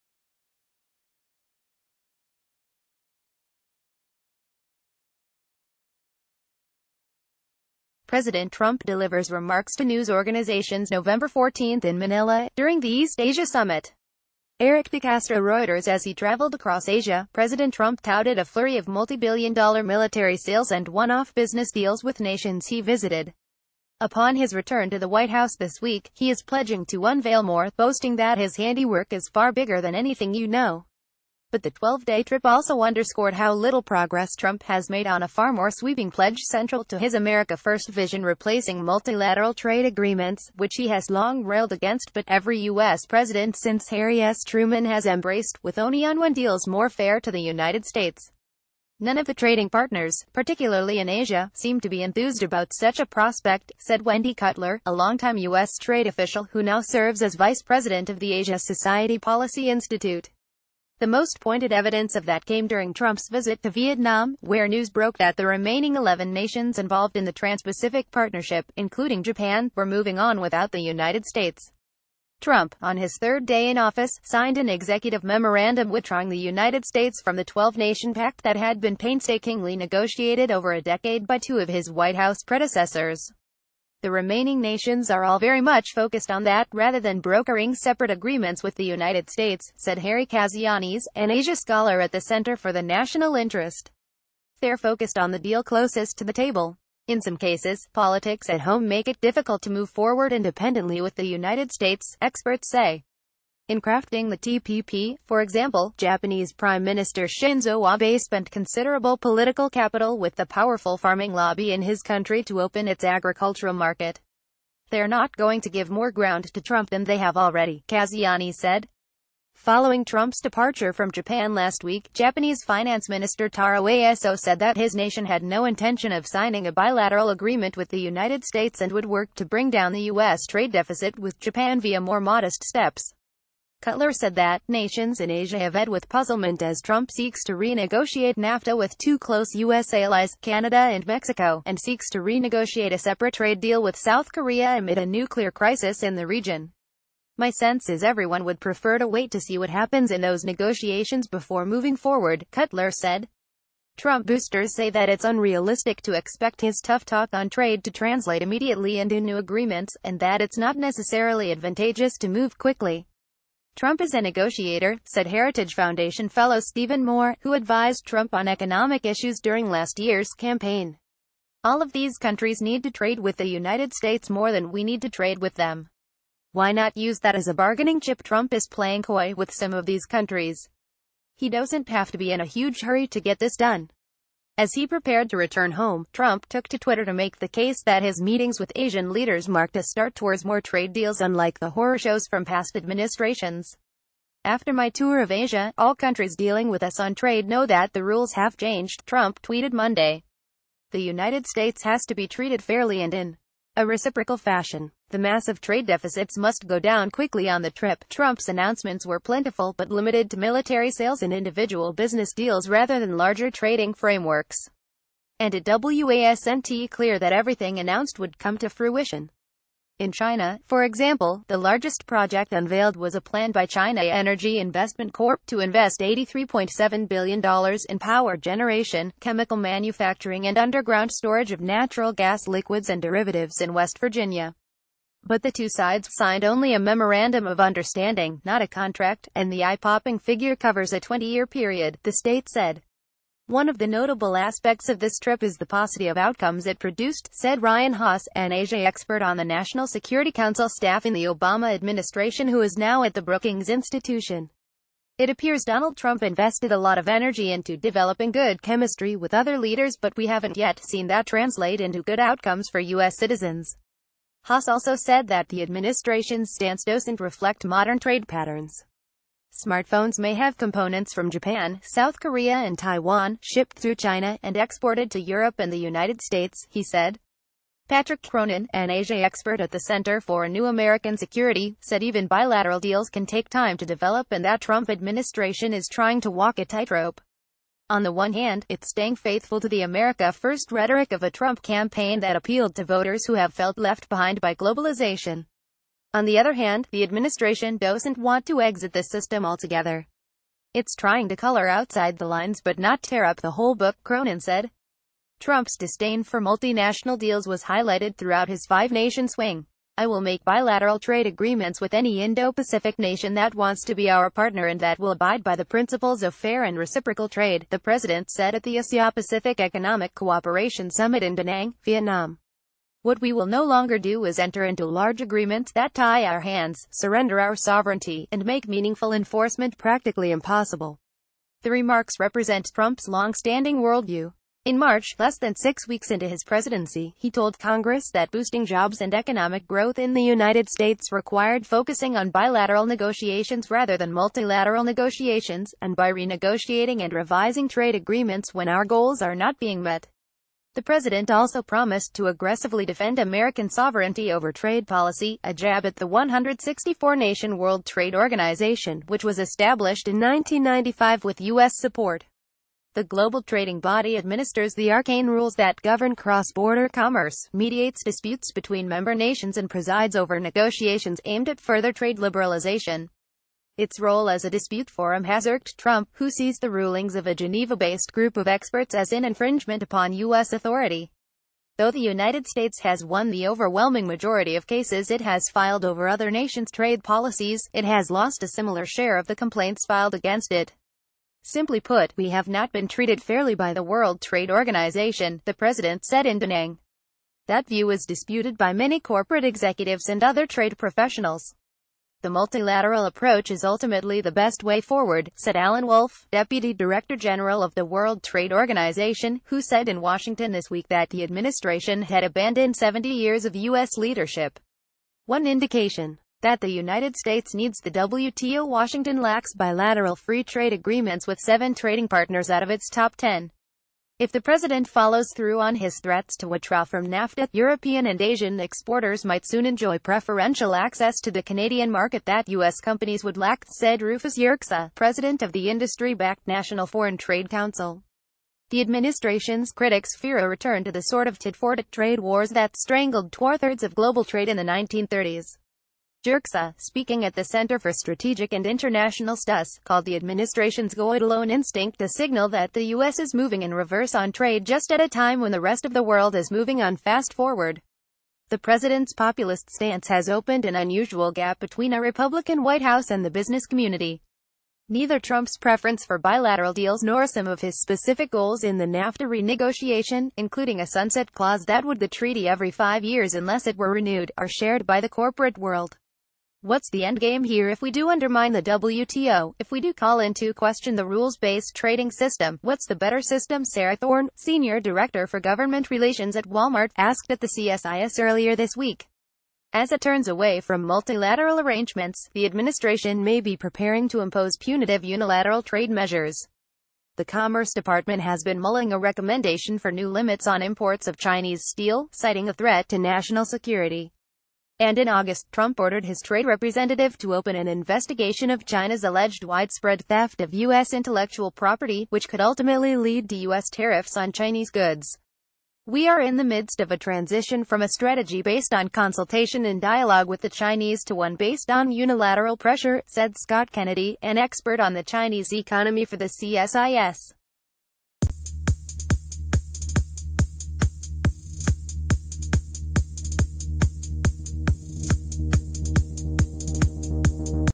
Trump said he would strike one-on-one trade deals. that’s not happening.
President Trump delivers remarks to news organizations Nov. 14 in Manila, during the East Asia Summit. Erik De CastroReuters As he traveled across Asia, President Trump touted a flurry of multibilliondollar military sales and oneoff business deals with nations he visited. Upon his return to the Whit...